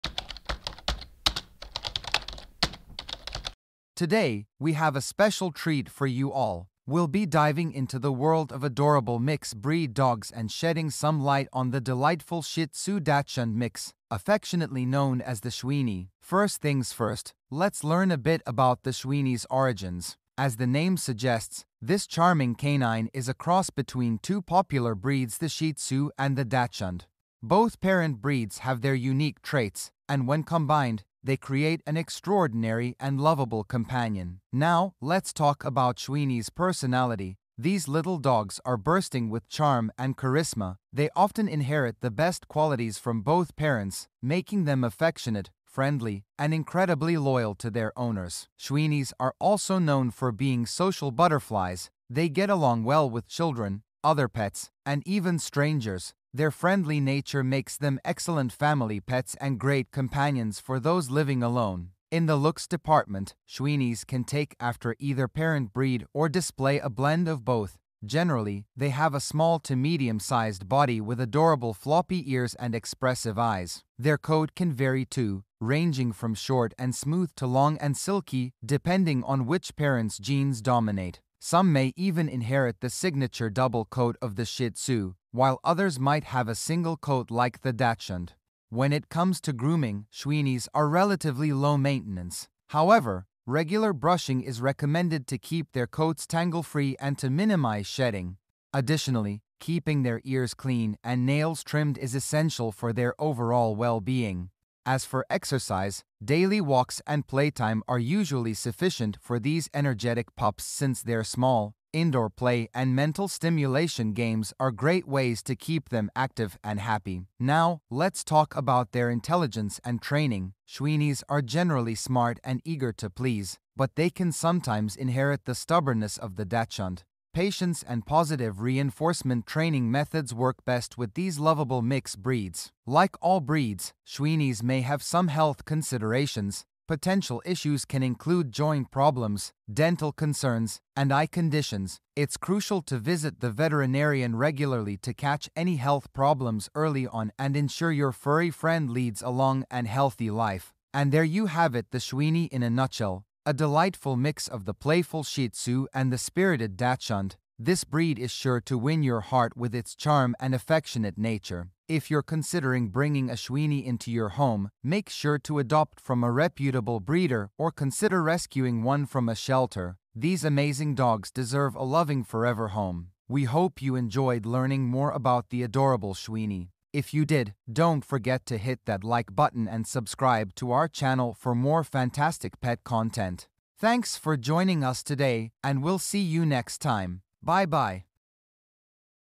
Shih Tzu Dachshund Mix aka Schweenie
Welcome to our comprehensive guide on the captivating Shih Tzu Dachshund Mix, a delightful crossbreed that combines the spirited personality of the Shih Tzu with the charming traits of the Dachshund. In this video, we delve into the unique characteristics, care tips, and fascinating facts about this lovable hybrid.

Video Content:
In this engaging video, we explore the captivating world of the Shih Tzu Dachshund Mix, uncovering a wealth of insights that every dog enthusiast should know:

Origins and Heritage: Discover the fascinating history and background of the Shih Tzu Dachshund Mix, tracing its lineage and the purposes for which it was initially bred.
Physical Appearance: Dive into the distinctive features and charming attributes that make this breed stand out, from its silky coat to its compact size.
Temperament and Personality: Learn about the endearing personality traits of the Shih Tzu Dachshund Mix, including its playfulness, loyalty, and affectionate nature.
Training and Care: Get valuable tips on how to best care for and train your Shih Tzu Dachshund Mix, ensuring a happy and harmonious relationship.
Health Considerations: Understand the potential health concerns associated with this crossbreed and how to proactively manage their well-being.